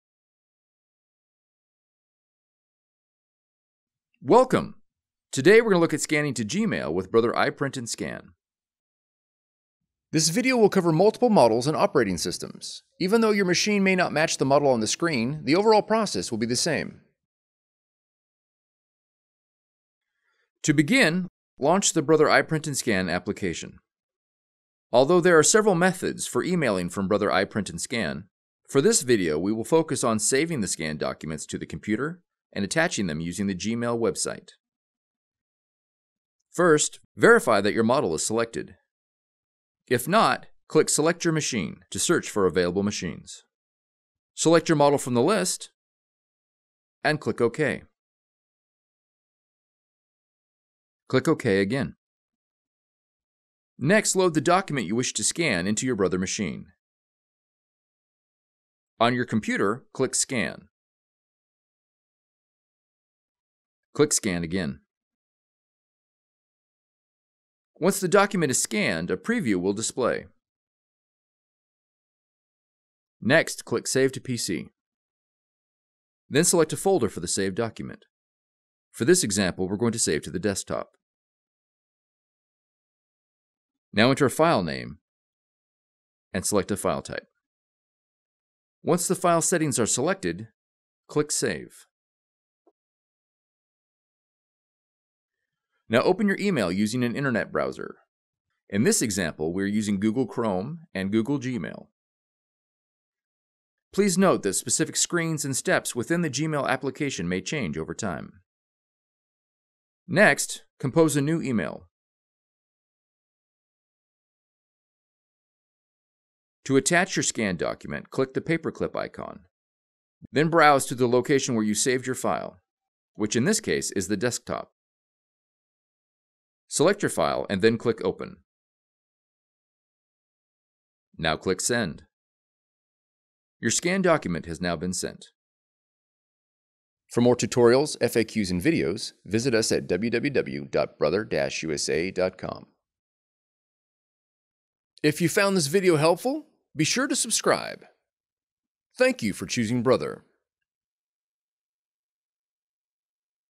Scan to Gmail from Brother iPrint&Scan
Learn how to scan documents to Gmail with Brother iPrint&Scan in this tutorial.

Follow this link to verify your model is compatible with Brother iPrint&Scan

Model shown: MFC-J995DW

Quick Links / Table of Contents:
Launch iPrint&Scan 00:21
Verify your model 00:36
Load document 00:53
Save document 01:07
Open Gmail 01:35
Attach scanned document 1:57
Send Email 2:13

Transcript:
Welcome. Today we are going to look at Scanning to Gmail with Brother iPrint & Scan

To begin, Launch the Brother iPrint & Scan application.

Although there are several methods for emailing from Brother iPrint and Scan, for this video we will focus on saving the scanned documents to the computer and attaching them using the G-Mail website.

First verify that your model is selected.  If it is not, click "Select your Machine", to search for available machines.

Select your model from the list, and click OK.

Next load the document you wish to scan into your Brother machine.  On your computer click Scan.

Click Scan again

Once the document is scanned, a preview of the document will display.

Next click on Save to PC. Then select a folder for the saved document.  For this example, we are going to save the desktop. Now enter a file name and select a file type.

Once all the file settings are selected click Save.

Now open your email using an internet browser.  In this example, we are using Google Chrome and Google Gmail. Please Note that the specific screens and steps within the Gmail application may change over time.

Next, compose a new email. To attach your scanned document, click the paper clip icon, then browse over to the location where you saved your file.  Which in this case is the desktop.

Select your file and then click open.

Now click Send.

Your scanned document has now been sent.

Customers also searched for:
Brother scan to gmail
iPrint and Scan to gmail